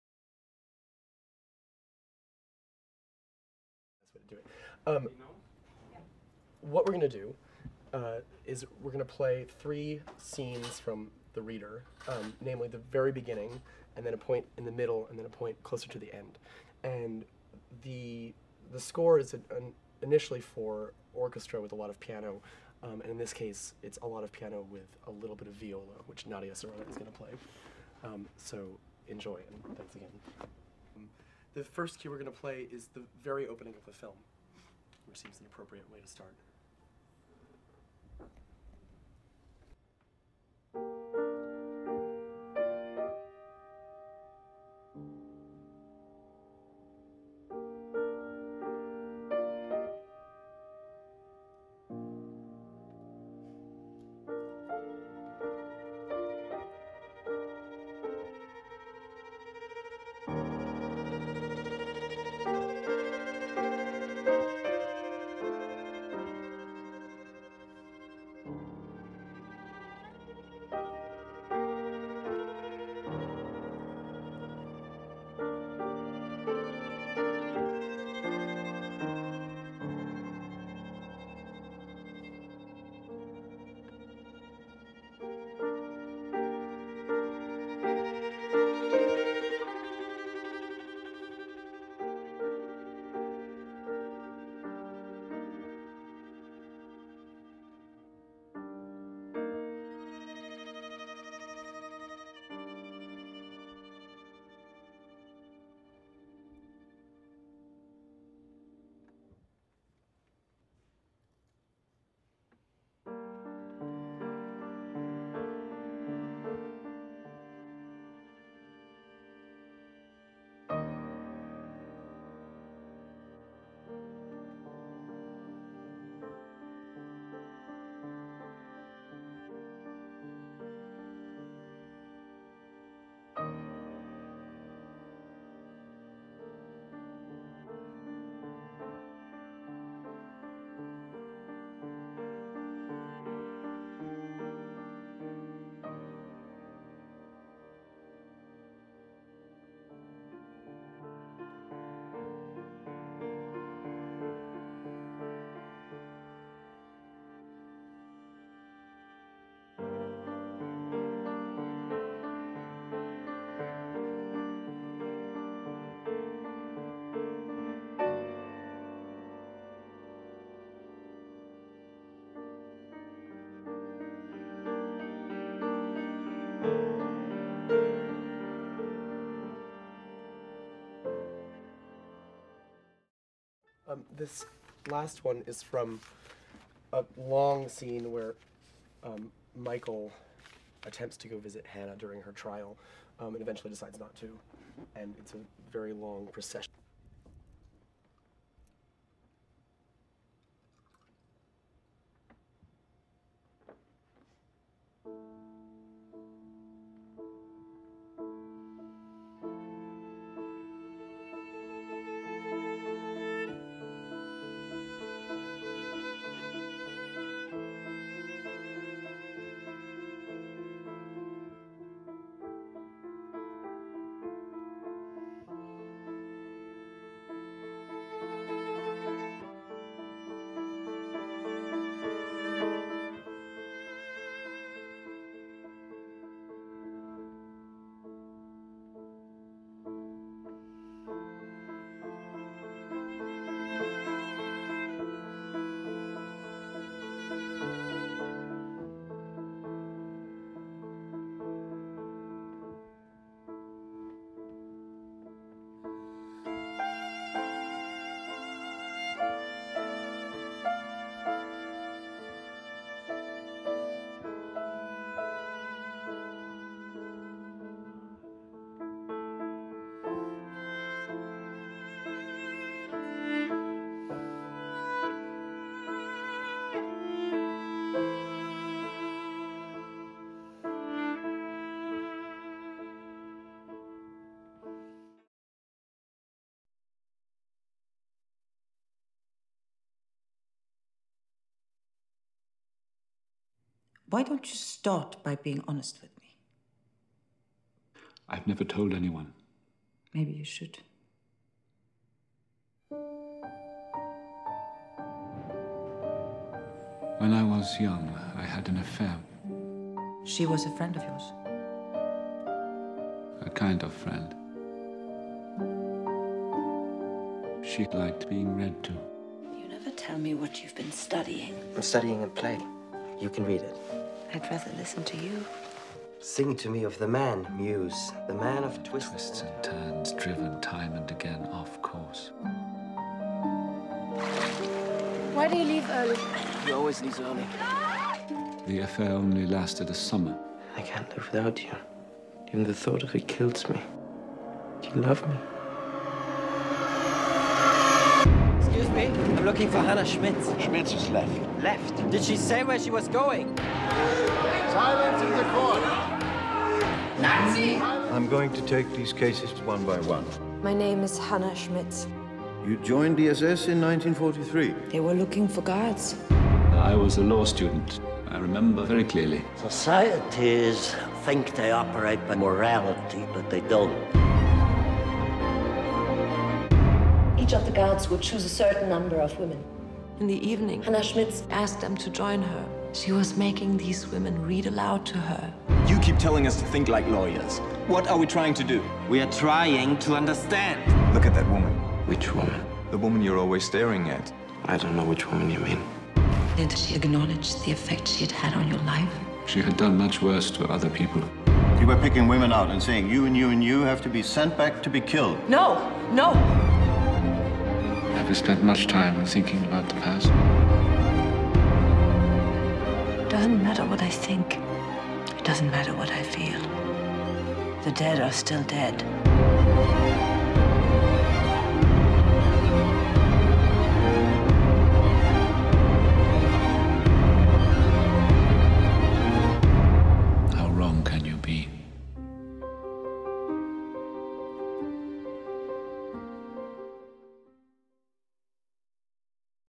The Reader Composer Nico Muhly at Cafe Carlyle
The composer from the Golden Globe nominated movie, THE READER performs at Cafe Carlyle with Nadia Sirota on viola. New York City - 12/10.  See THE READER in select theaters 12/12 and nationwide 12/25.